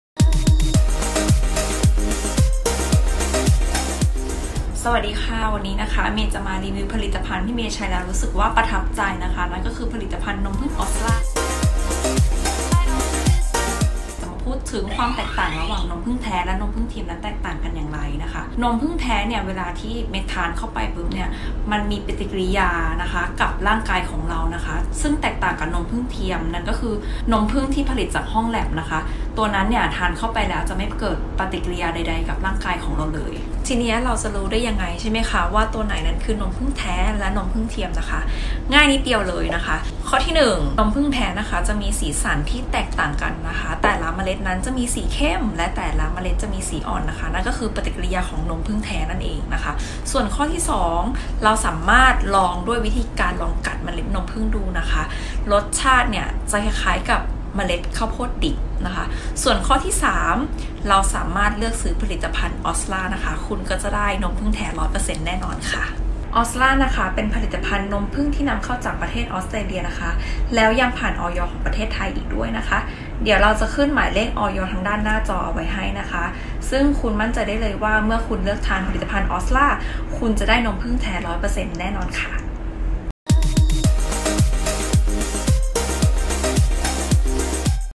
นมผึ้งออสลา AUSLA ROYAL JELLY
รีวิวผลิตภัณฑ์ นมผึ้งออสลา
โดย เมย์ จากประเทศออสเตรเลีย